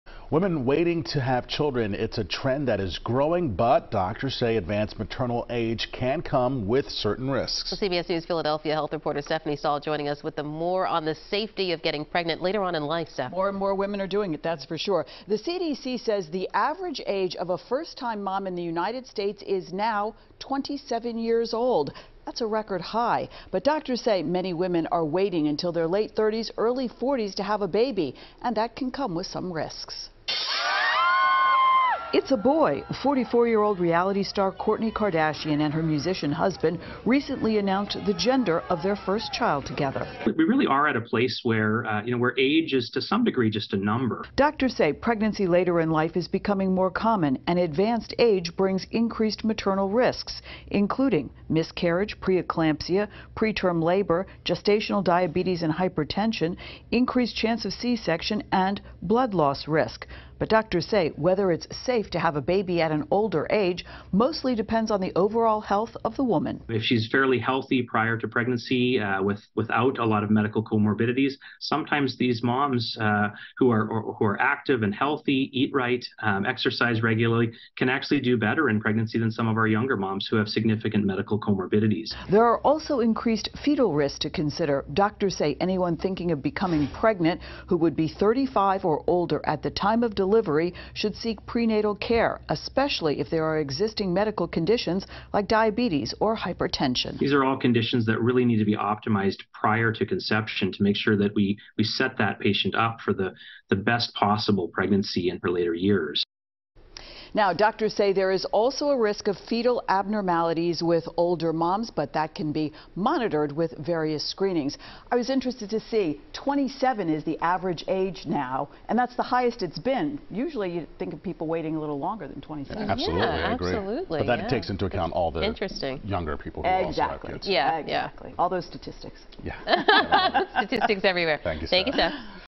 Advanced maternal age pregnancy can come with risks, doctors say
Doctors say more women are waiting until their late 30s or early 40s to have a baby and that can come with some risks.